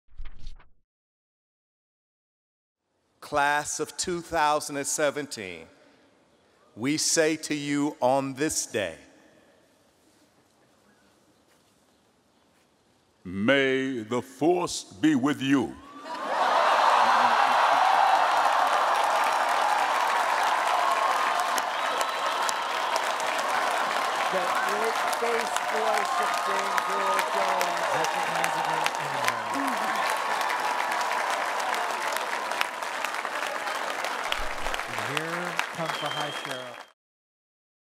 James Earl Jones tells Harvard grads "May the Force be with you"
Legendary actor and Harvard honorary degree recipient James Earl Jones shares the famous Star Wars blessing with the 2017 graduates of Harvard University